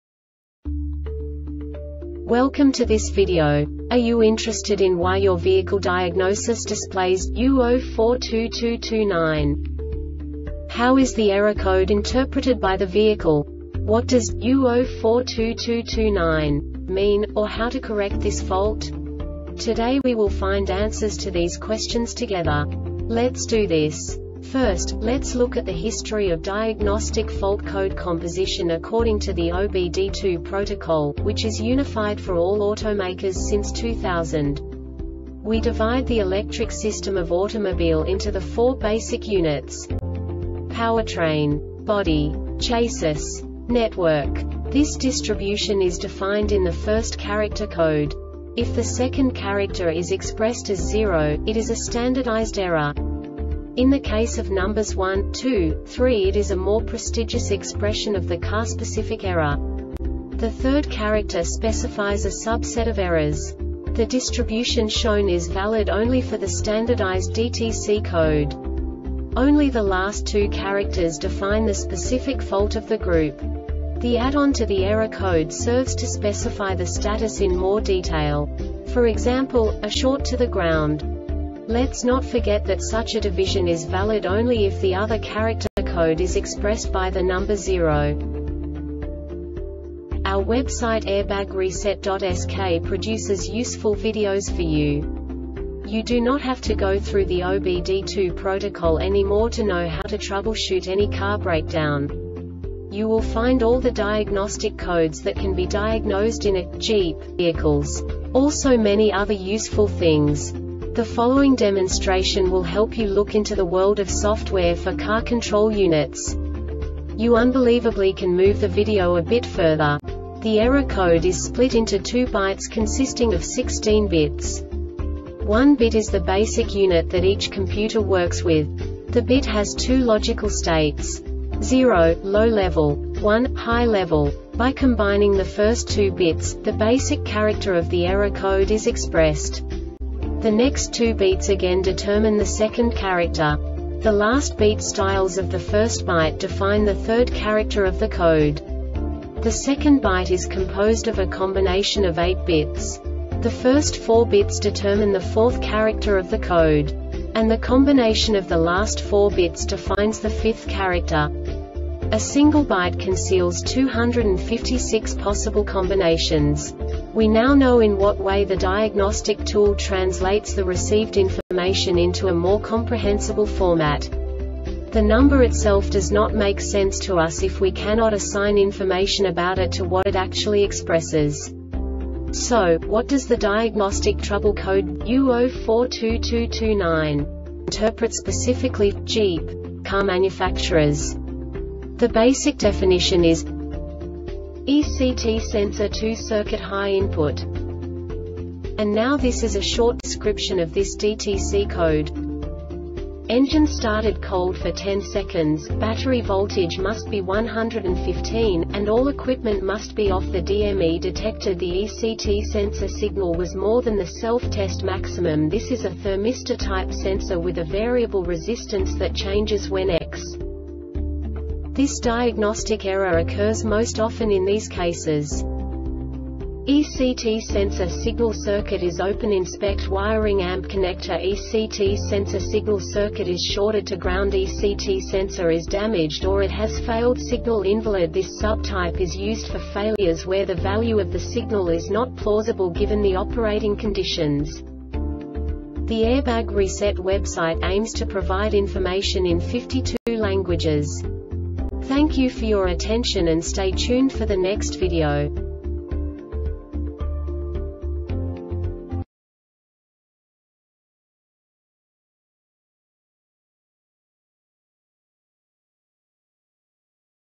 DTC Jeep U0422-29 Short Explanation
Contents:
0:21 Basic DTC analysis according to OBD2 protocol standard.
1:48 Insight into programming
2:58 A diagnostically understandable description of the Diagnostic Trouble Code
3:05 Basic error definition

Make
Jeep
Code
U0422 29
Definition
ECT Sensor 2 Circuit High Input
Description
Engine started cold for 10 seconds,  battery voltage must be 115, and all equipment must be off  The DME detected the ECT sensor signal was more than the self-test maximum  This is a thermistor-type sensor with a variable resistance that changes when ex
Cause
ECT sensor signal circuit is open inspect wiring  amp  connectorECT sensor signal circuit is shorted to groundECT sensor is damaged or it has failed
Failure Type
Signal Invalid